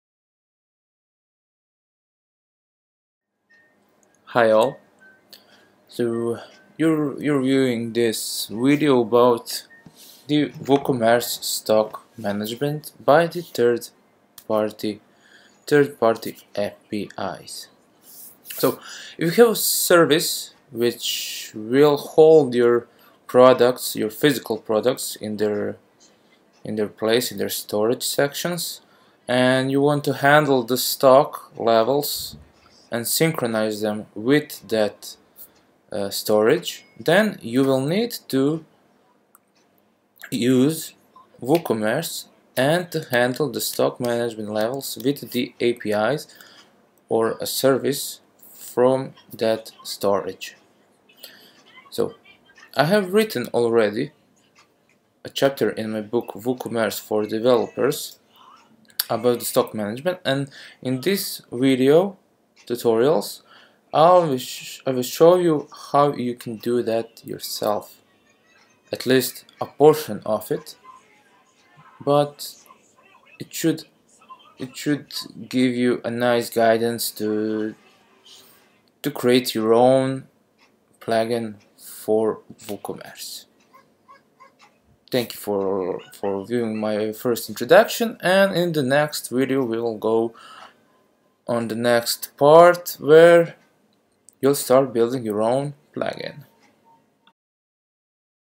WooCommerce Stock Managemenet - Introduction
Learn how to manage your Stock levels in WooCommerce when using 3rd party APIs or Services.